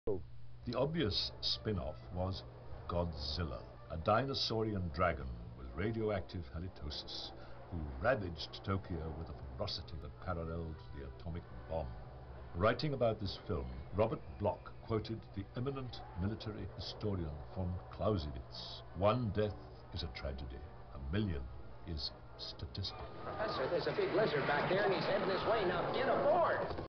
Christopher Lee on Godzilla
Christopher Lee talks briefly about Gojira 1954